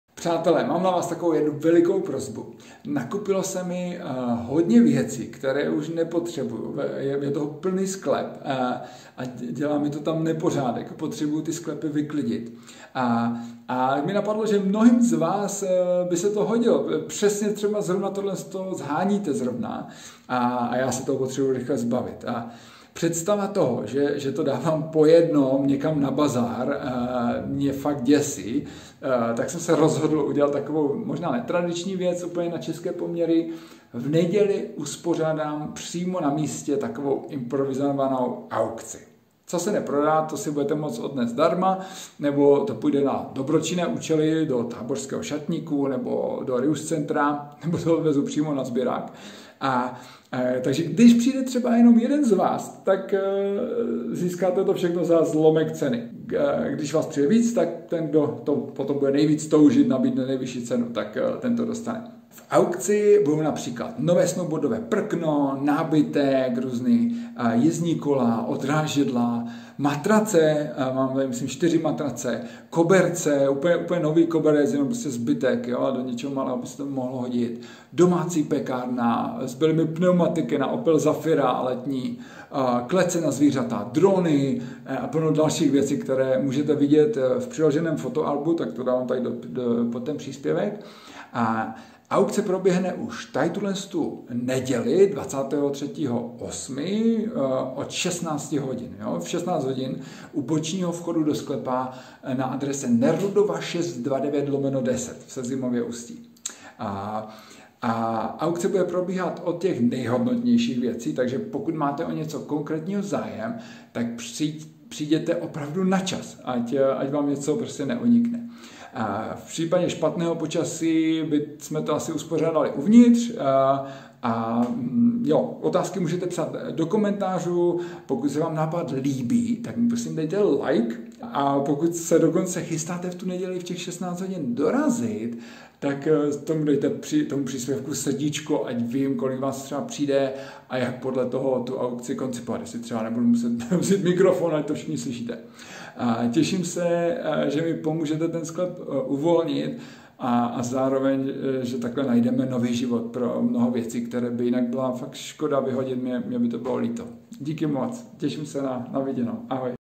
Veřejná aukce - v neděli 23.7. v 16:00 na adrese Nerudova 629/10, Sezimovo Ústí.
Přátelé, nakupilo se mi hodně věcí, které již nepotřebuji a dělají mi ve sklepě nepořádek. Ale třeba mnozí z vás přesně toto sháníte. Potřebuji se toho rychle zbavit a představa, že to dávám po jednom někam na bazar mě děsí. Rozhodl jsem se proto v neděli uspořádat přímo na místě takovou improvizovanou aukci. Co se neprodá si budete moct odnést zdarma, nebo to půjde na dobročinné účely Táborského šatníku nebo do Táborského Re-use centra či přímo na sběrný dvůr. Když přijde jen jeden z vás, tak získáte vše za zlomek ceny. Když vás přijde více, tak ten, kdo nabídne nejvyšší cenu, věc dostane. V aukci budou například nové snowboardové prkno, nábytek, jízdní kola a odrážedla, matrace, koberce, domácí pekárna, pneumatiky na Opel Zafira, klece na zvířata, drony, a plno dalších věcí, které můžete vidět v přiloženém foto albu. Aukce proběhne už tuto neděli 23.8. v 16:00 u bočního vchodu do sklepa na adrese Nerudova 629/10, kde tyto věci jsou. Aukce bude probíhat od těch nejhodnotnějších věcí, takže pokud máte o něco konkrétního zájem, přijděte opravdu načas, ať vám něco neunikne. V případě špatného počasí aukce proběhne uvnitř. Otázky napište do komentů a pokud se vám nápad líbí, dejte mu like, a pokud se dokonce chystáte v neděli v těch 16 hodin dorazit, dejte příspěvku srdíčko, ať vím, kolik vás přide a jak podle toho aukci koncipovat. Těším se, že mi pomůžete uvolnit sklep a zároveň najít nový život pro mnoho věcí, které by jinak byla škoda vyhodit. Díky